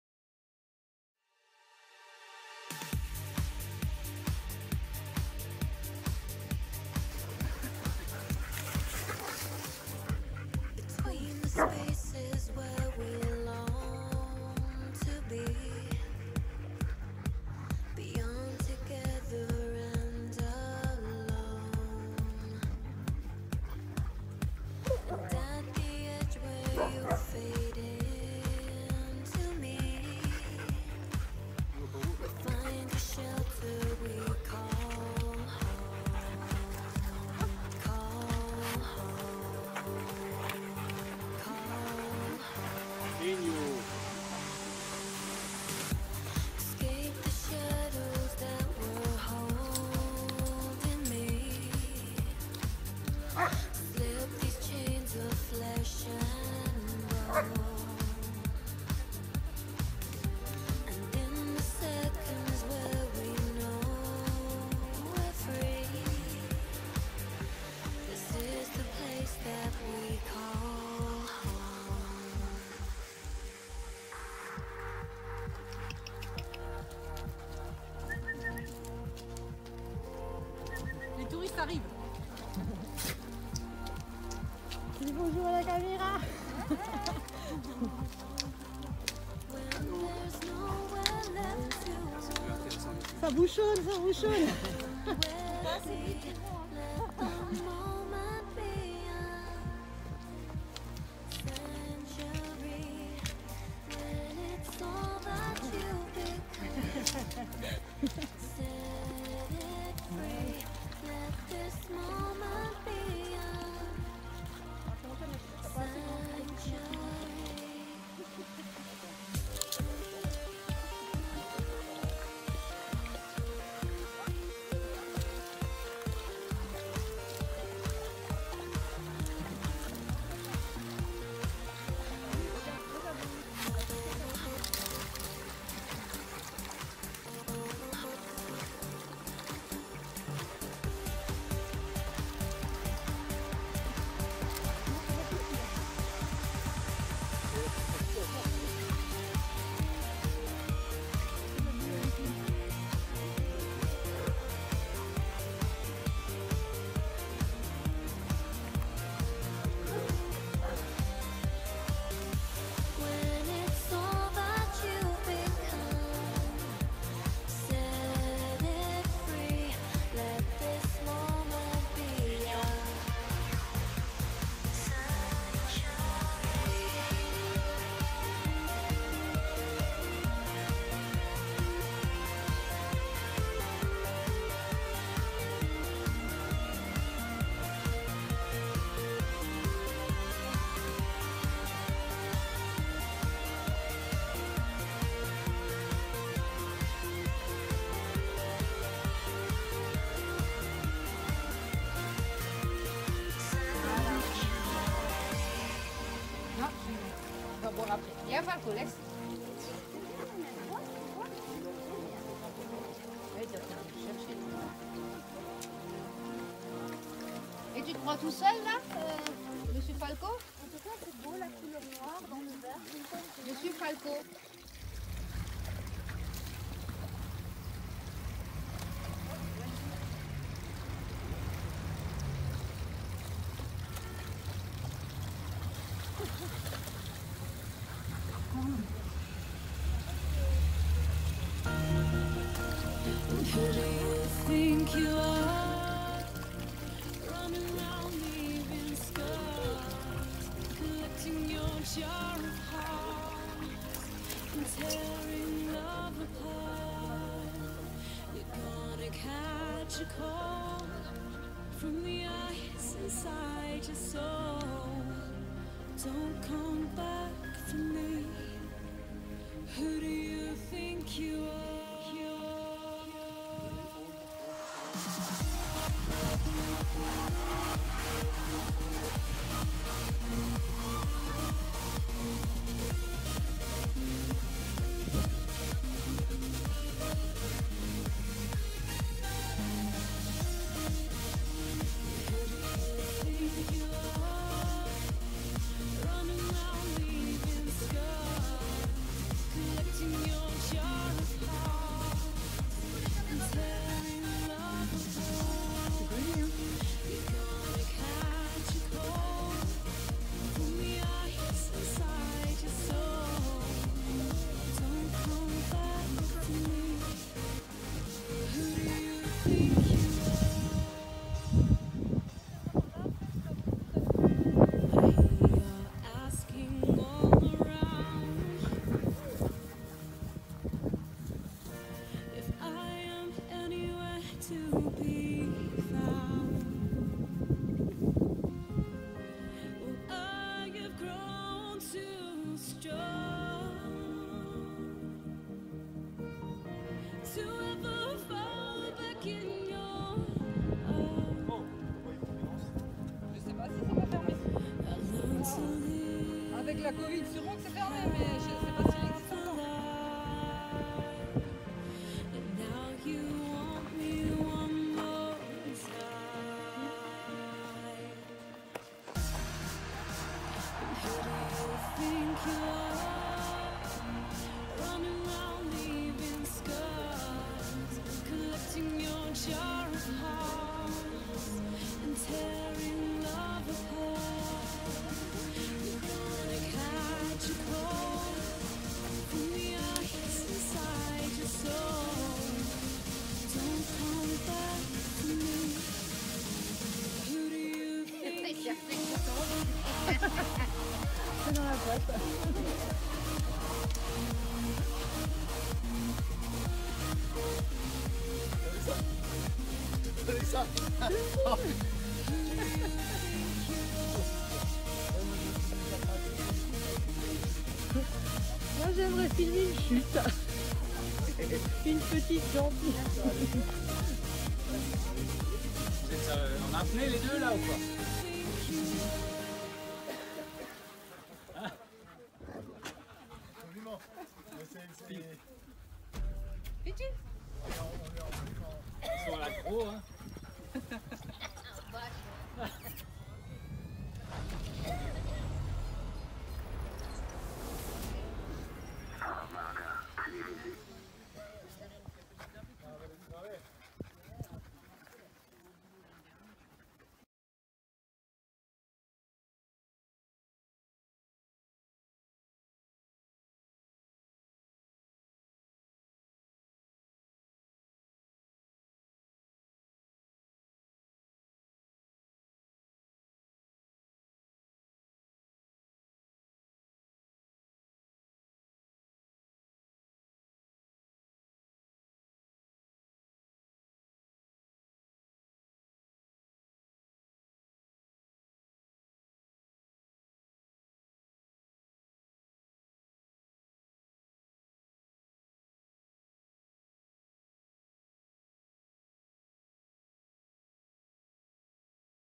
Randonnée dans les Vosges
Belle randonnée avec les Loulous autour de la vallée de Murbach